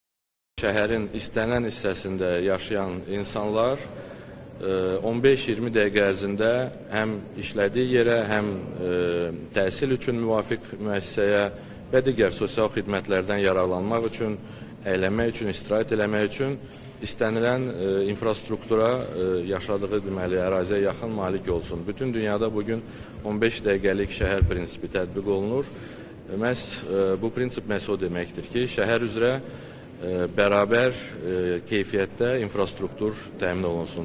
İnsanlar istədiyi yerə 15 dəqiqəyə çata biləcək
Bunu jurnalistlərə açıqlamasında Dövlət Şəhərsalma və Arxitektura Komitəsinin sədri Anar Quliyev Bakı şəhərinin 2040-cı ilədək inkişafına dair Baş planının təqdimatı mərasimində bildirib.